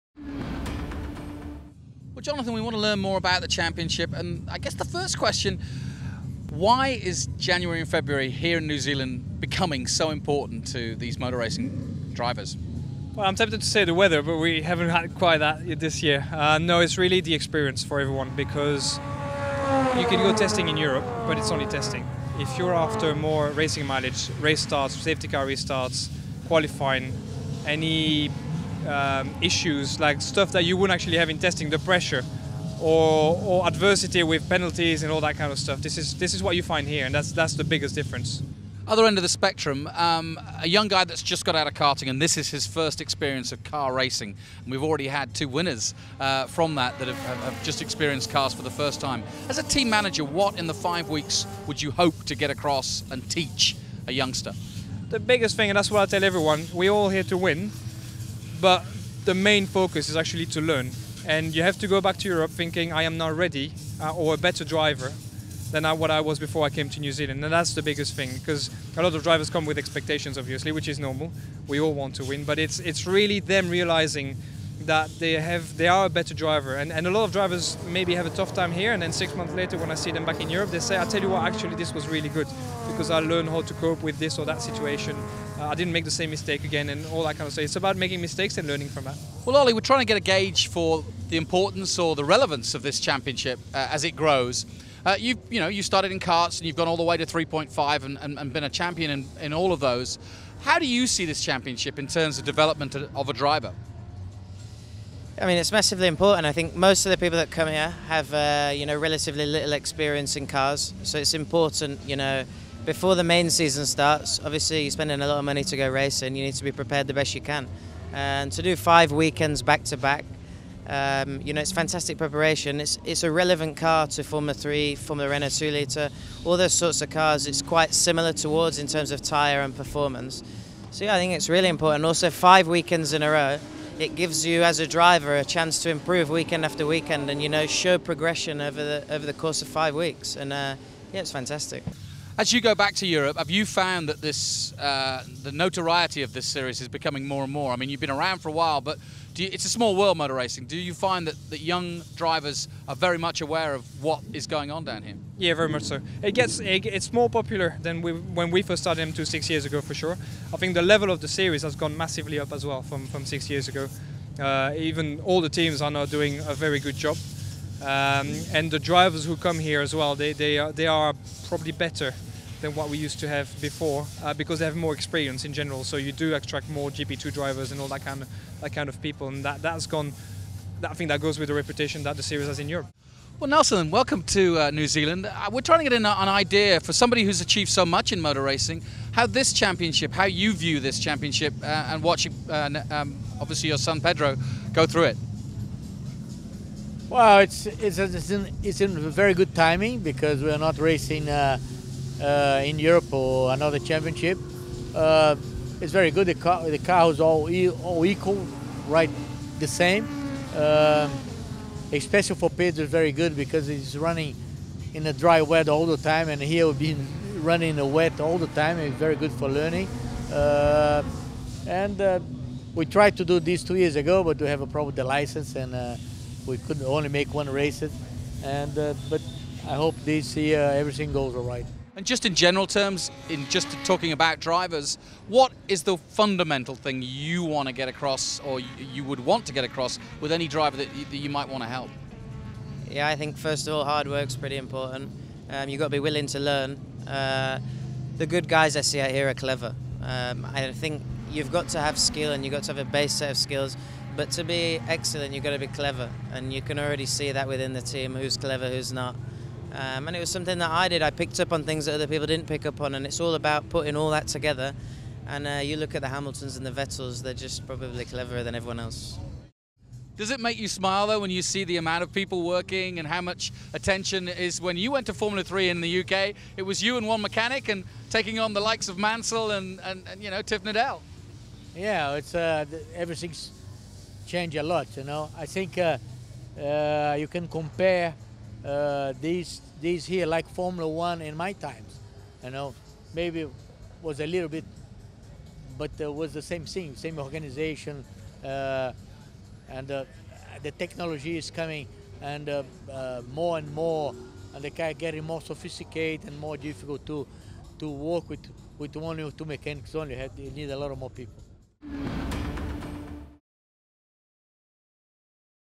WHY RACE IN THE TOYOTA RACING SERIES?
3 time F1 champion Nelson Piquet,  3.5 champion Ollie Rowland and m2 competition team Manager Jonathan Rowland discuss why the Toyota racing series is now a must do for aspiring F1 hopefuls